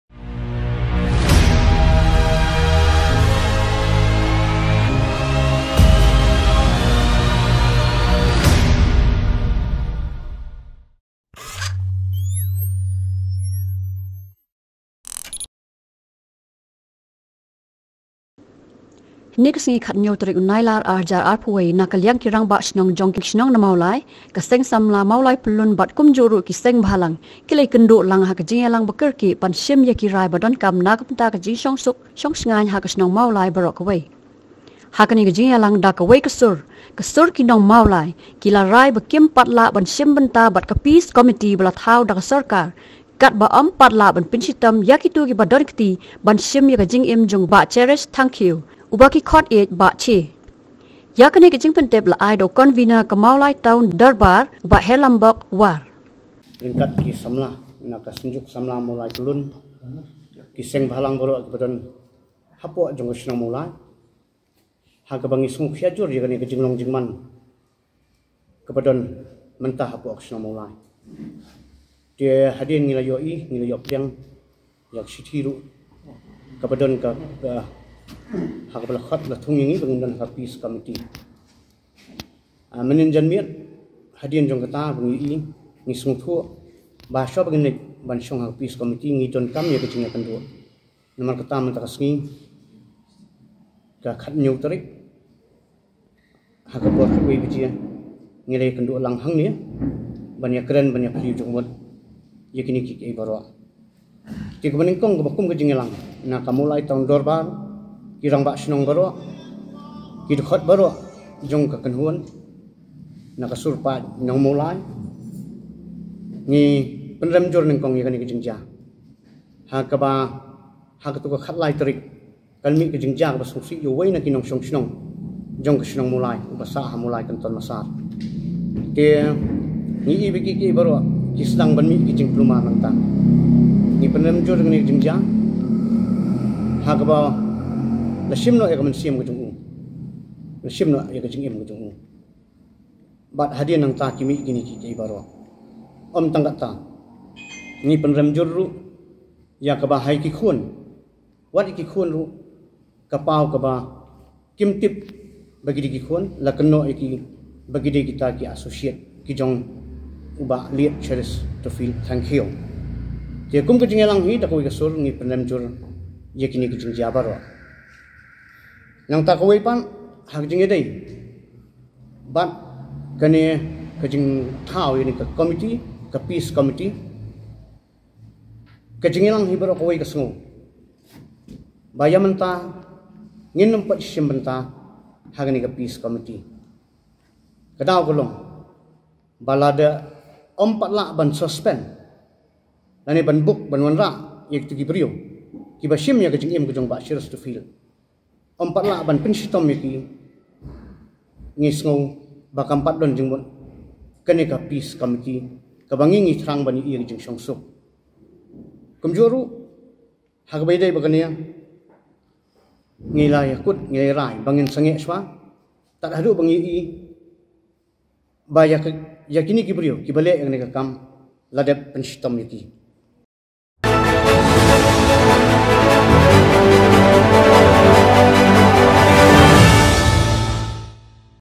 YM PAT TREH SHIM BYNTA NONGMAWLAI HAKA PEACE COMMITTEE JONG KA SORKAR JYLLA
Ka Sur Ki Nong Mawlai, ki la rai ba kim pat lah ban shim bynta bad ka Peace Committee ba thaw da ka sorkar, katba ym pat lah ban pynshitom ia kito ki ba donkti ban shim ia ka jingim jong u bah Cherish Thangkhiew, u ba ki khot ieid Bah Che. Ia kane ka jingpyntip la ai da u Convenor ka Mawlai Town Durbar, u bah Herlambok War.